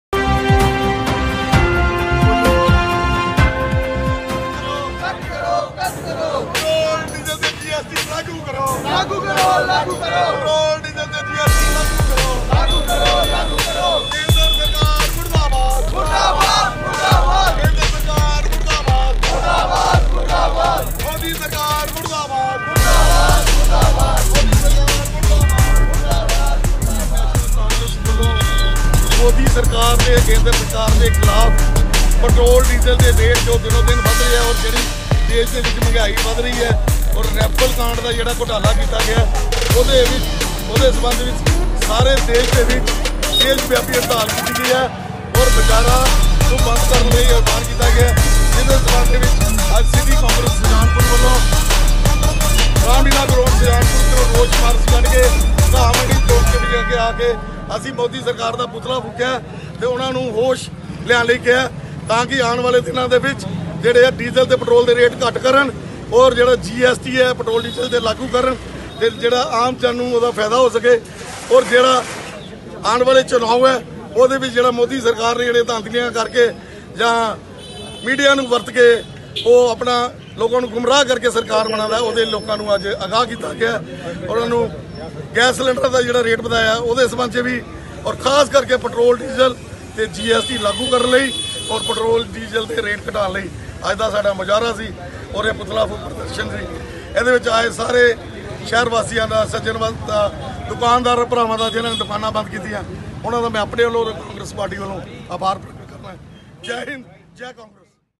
पेट्रोल-डीजल के मूल्यों को भी जीएसटी के दायरे में लाए केन्द्र सरकारः विनय महाजन ......
पेट्रोल-डीजल के दामों में बढ़ोत्तरी को लेकर सुजानपुर में कांगे्रस ने फूंका पीएम का पुतला.